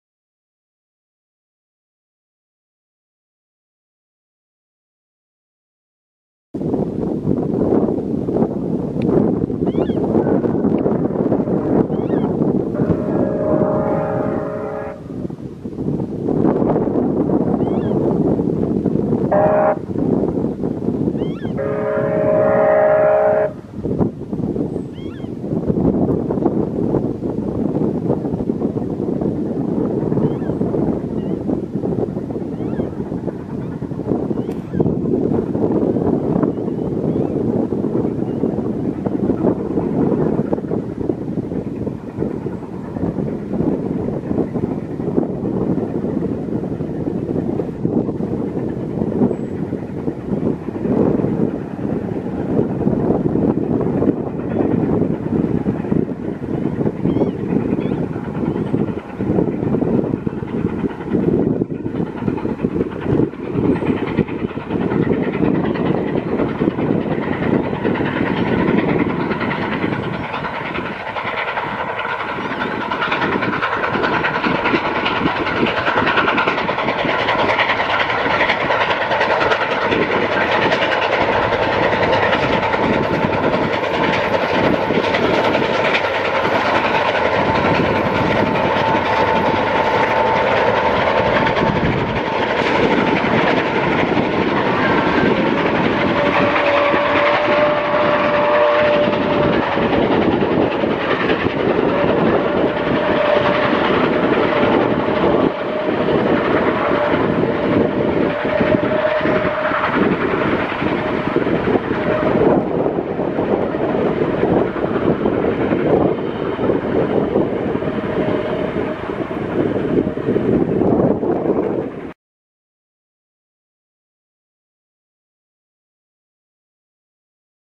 CP 2816 eastbound near Indus, AB. (April 26/24).
Here we see CP 2816 leading its train east near Indus, AB. It has begun its Final Spike Tour which will end in Mexico. The tour is to commemorate the 1 year anniversary of the CP/KCS merger.